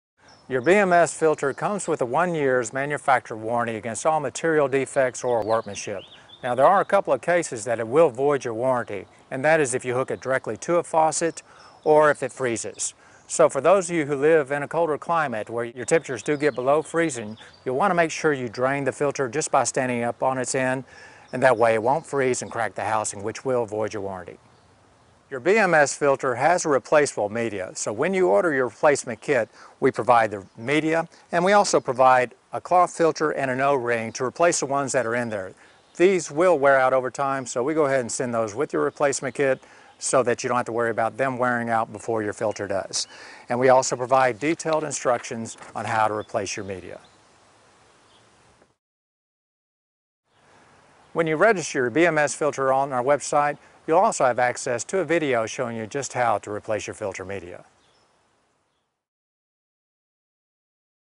The Filter of Eden Warranty
This video describes what the Filter of Eden warranty covers and what not to do that will void the warranty.